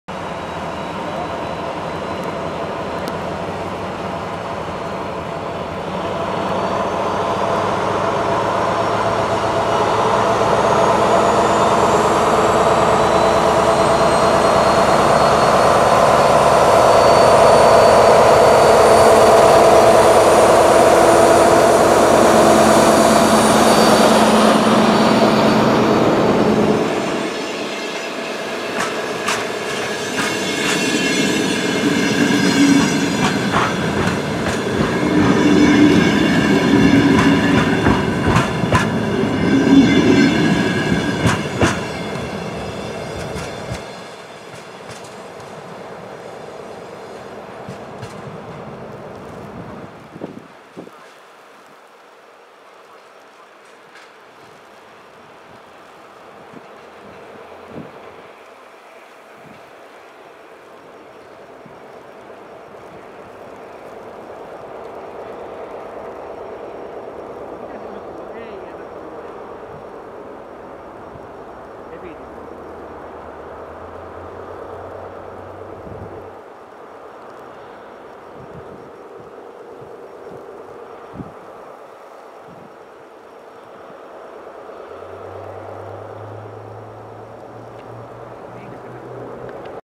H 761 lähtee Joensuusta lumipyryssä
Katso parhaita rataosavideoita:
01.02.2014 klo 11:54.
H 761 Nurmekseen lähtee Joensuusta. Juna odotti myöhässä olleen Ic 1:n matkustajia. Dv12 2516.